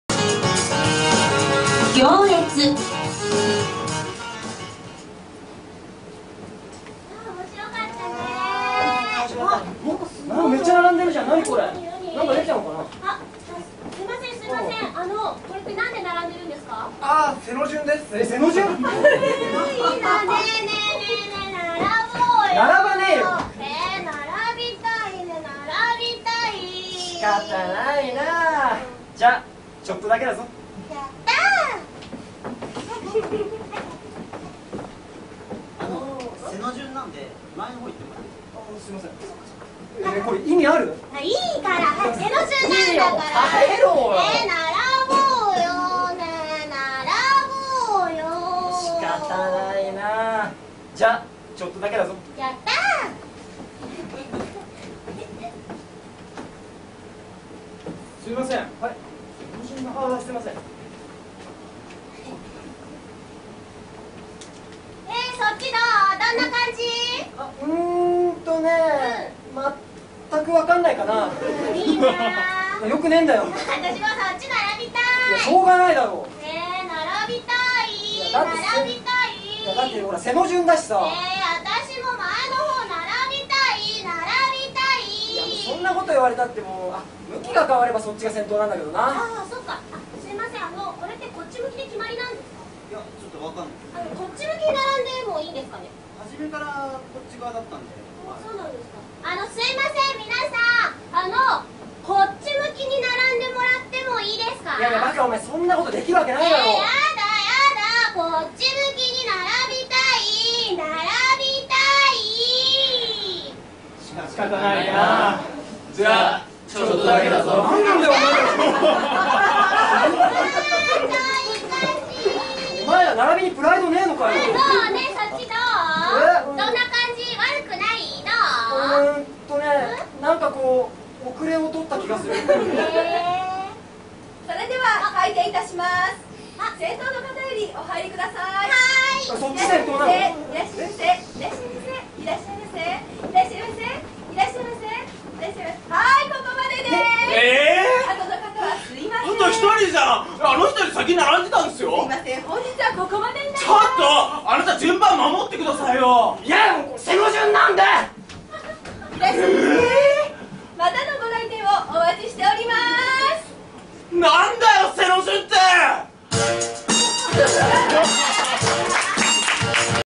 『行列』 IKKANがいろいろなコントを作るライブ
『行列』

【作／演出】
IKKAN

【出演】
彼氏　シャイン内之宮
彼女　まーみ
男１　ぬきやま
男２　西村三等兵
男３　ガッツ古川
男達　松本一平
男達　真咲逞
男達　リッキー近野
男達　Naoya-K
男達　ばばスケ
男達　下地アングラ省吾
男達　グラッチェ小椋
店員　鳳恵弥

『IKKANがいろいろなコントを作るLIVE』

2014年10月19日（日）
18:00開場／18:30開演

【構成：演出】
IKKAN

【出演】
鳳恵弥
まーみ
松本一平
ぬきやま
真咲逞
椎名メグミ
リッキー近野
Naoya-K
ばばスケ
シャイン内之宮
西村三等兵
ガッツ古川
下地アングラ省吾
グラッチェ小椋

【会場】
ミニホール新宿Fu-
東京都新宿区歌舞伎町2-45-5　新宿永谷ビル1F